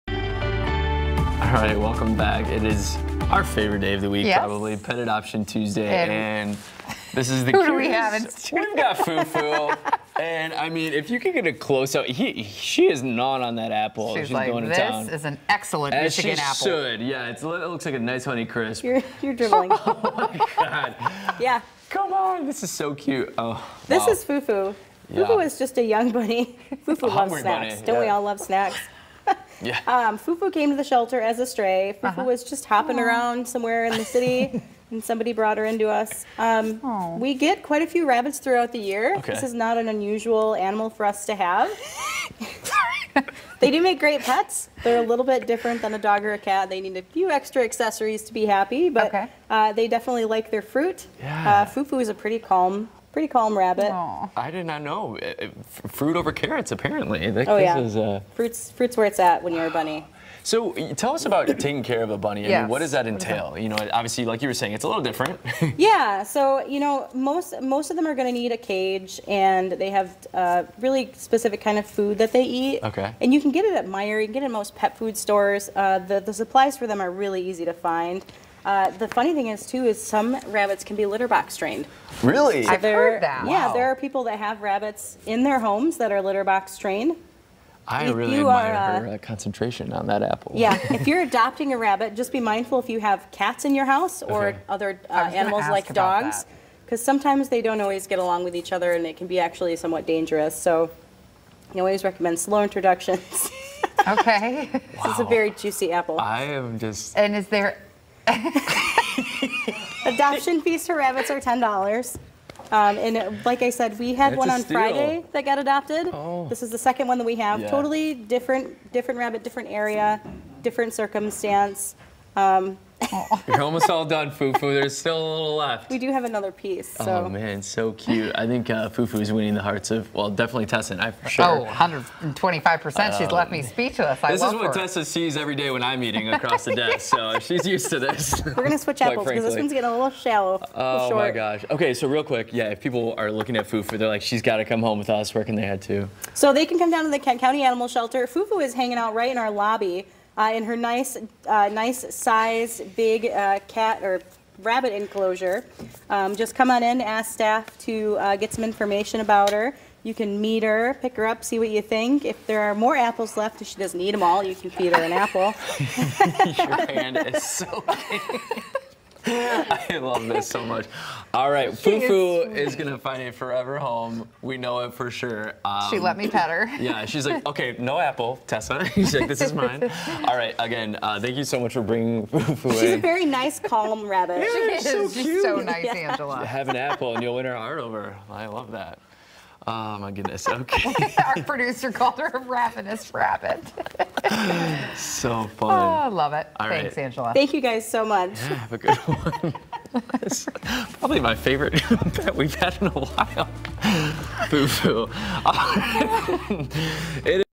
Pet Adoption Tuesday: Meet Foo Foo
Foo F00 is a young bunny who came to the shelter as a stray after someone found her hopping around the city. Now, she’s hoping to find her forever home.  Foo Foo is a calm bunny who loves to snack on apples.  According to Angela Hollinshead with the Kent County Animal Shelter, some rabbits can be trained to use a litter box.